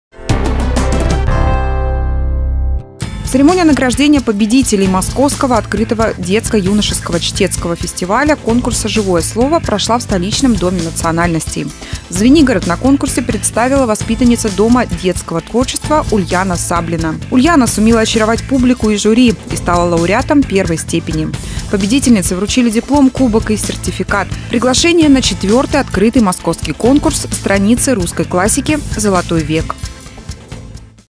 Живое слово
Жительница Звенигорода заняла первое место на фестивале-конкурсе чтецов